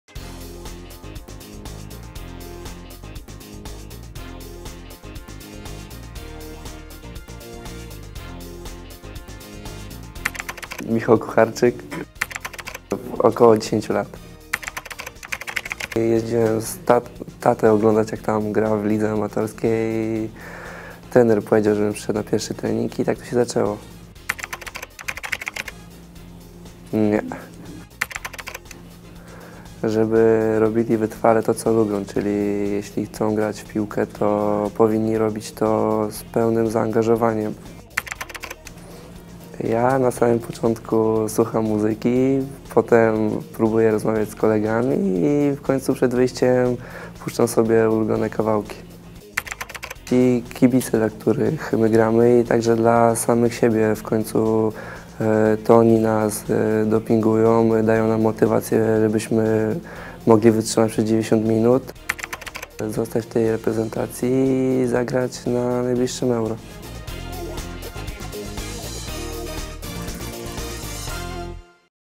Michał Kucharczyk - nieśmiały debiutant
Dzięki temu, że spodobała się Wam nasza seria filmików "Reprezentanci prywatnie" zdecydowaliśmy się na produkcję drugiej części naszego małego serialu:) Przepytaliśmy Michała Kucharczyka i innych zawodników m.in. o to, co lubią robić w wolnym czasie, jak mobilizują się przed meczami i jakie mają rady dla początkujących piłkarzy.. Obejrzycie, bo warto! Przed Wami pytania o tematyce sportowej, ale będą również i inne! Polecamy!